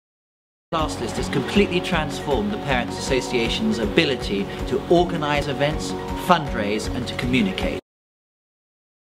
Richard Murray
Richard Murray, Head of Christ Church Cathedral School, How Classlist has helped transform their school community.